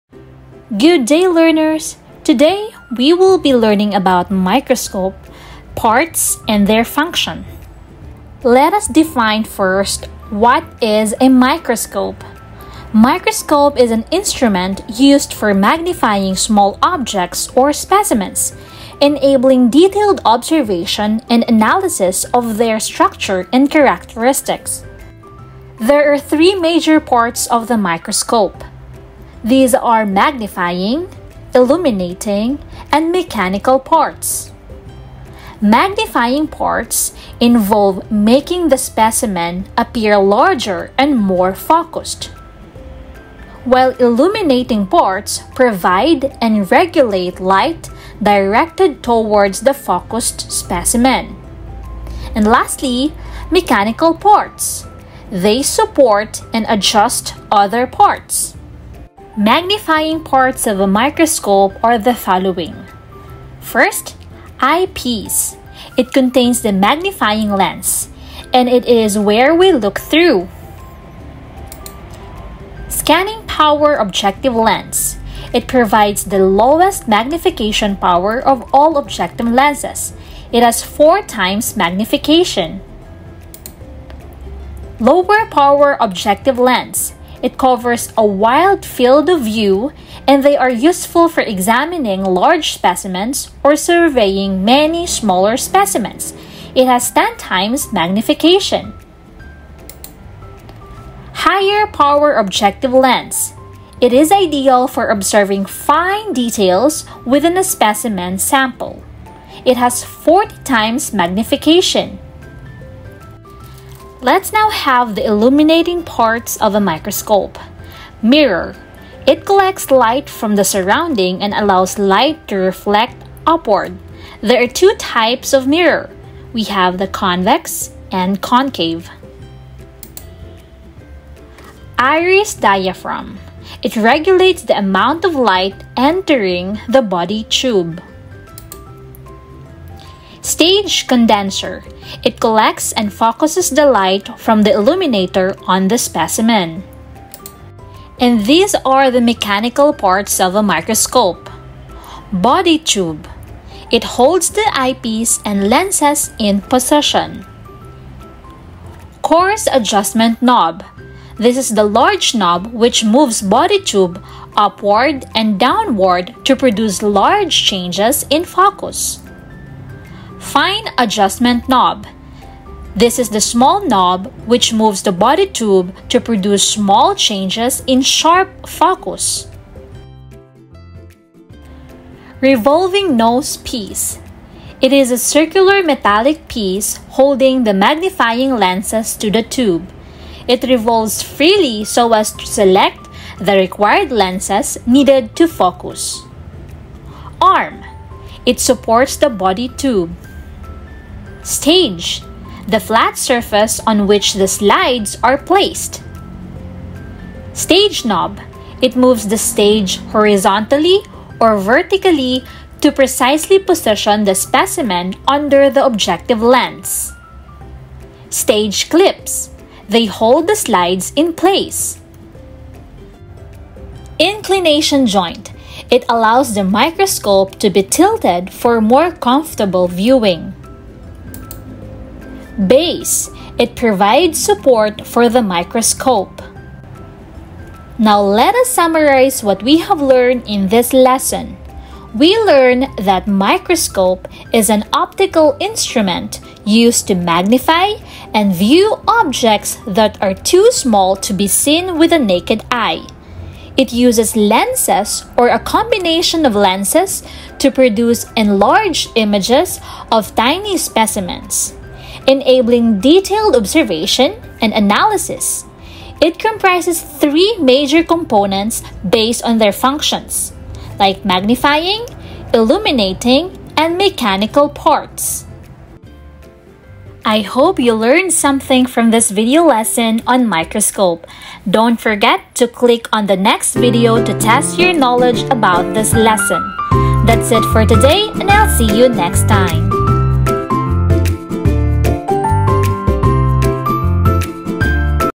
Microscope (Parts and Functions) | Grade 7 Science DepEd MELC Quarter 2 Module 1 UPDATED VERSION
In this video lesson, you will learn the different parts of the Microscope and their function.